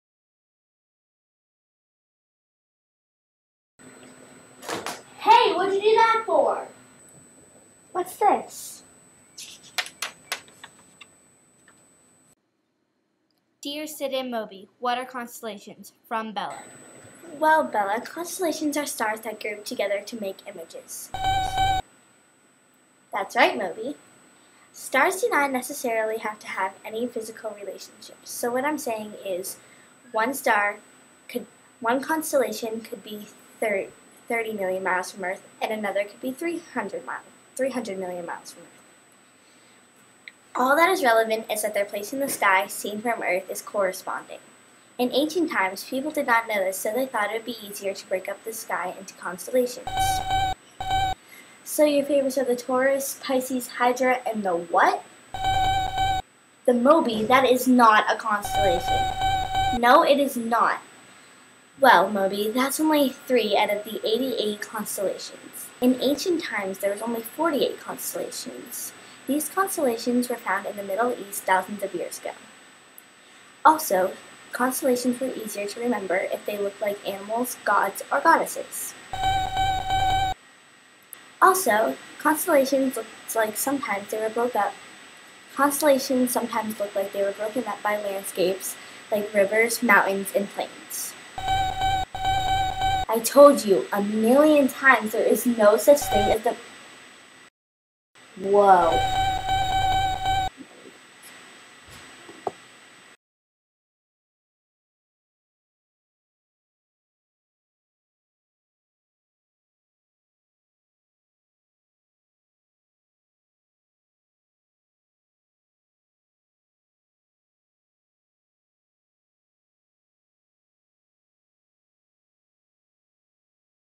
Constellations
Learn about constellations as Moby and Sidney elaborate on the formation, origins, and meanings of constellations.